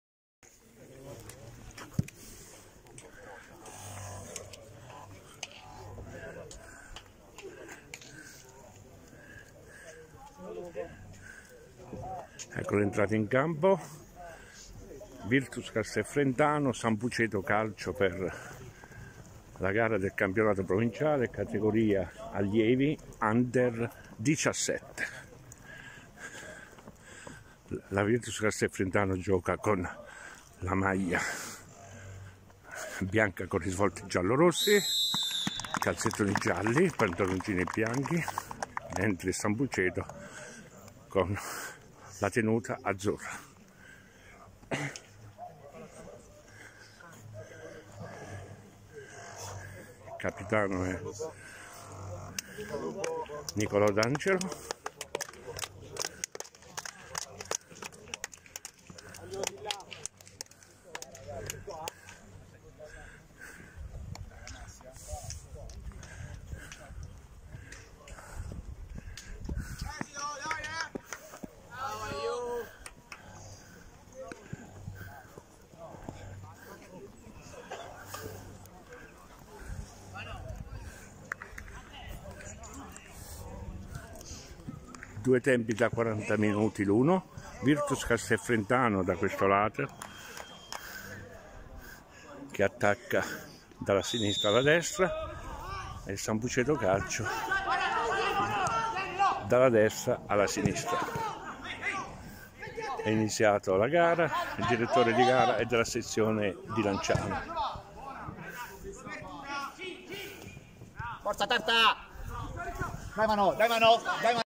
VIRTUS CASTEL FRENTANO CATEGORIA ALLIEVI UNDER 17 PROVINCIALI
ENTRATA IN CAMPO .
La gara di Sabato scorso , 25 Gennaio 2020 dei nostri Allievi di Mister Giovanni Di Giulio al " Nuovo " di Via Olimpia di Castel Frentano contro il Sambuceto Calcio .